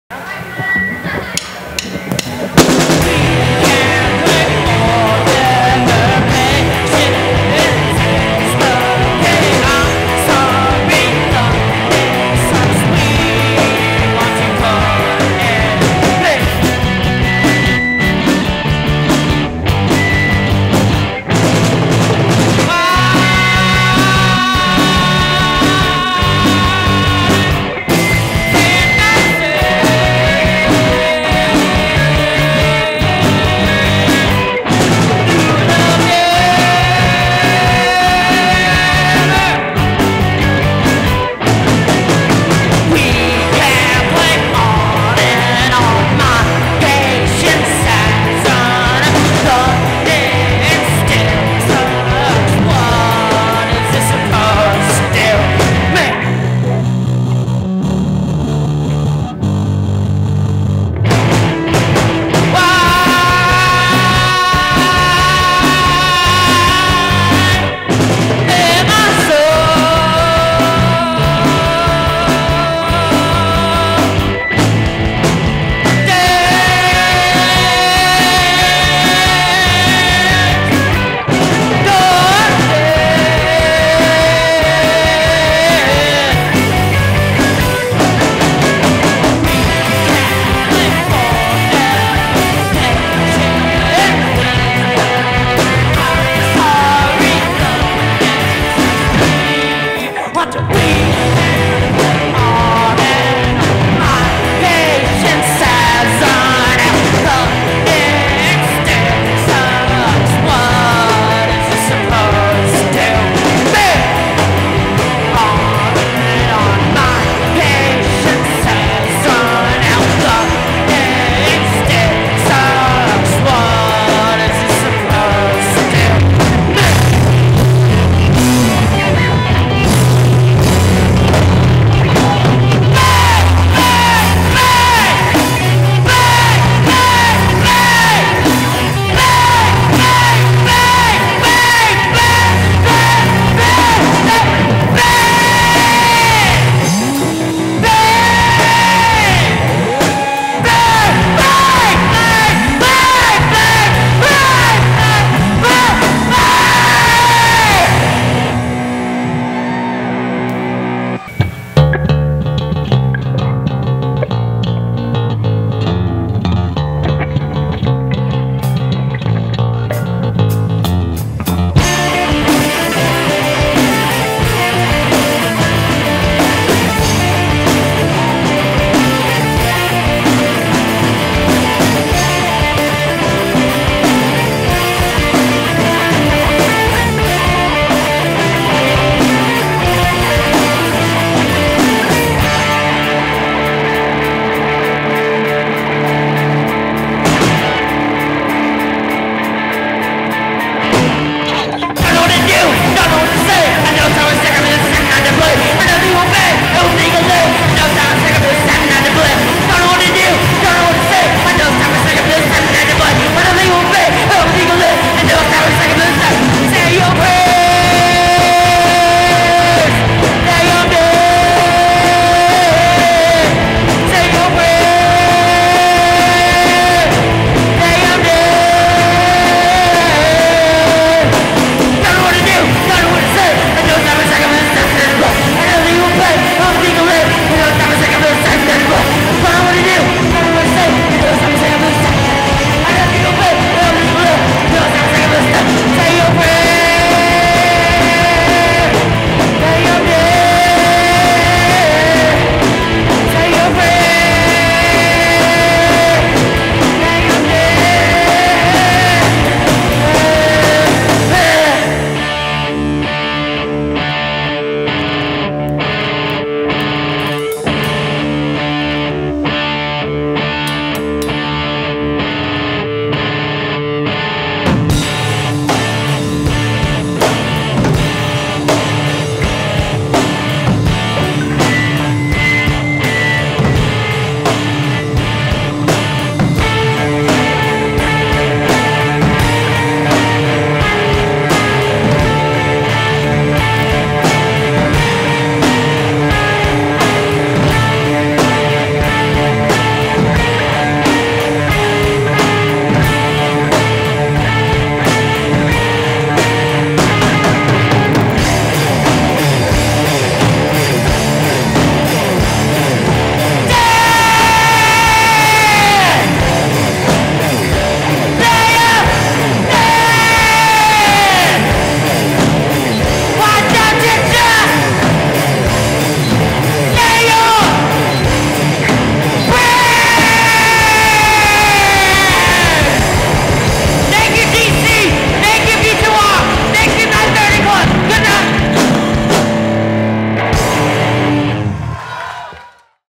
"Lay Em' Dead" - 2nd Place in Battle of the Bands VII (Maryland - Middle School Division)
"Lay Em' Dead" won 2nd place in Maryland's Middle School Division of the competition at B2R's Battle of the Bands VII at the 9:30 Club in Washington D.C. on Sunday, January 30th, 2011.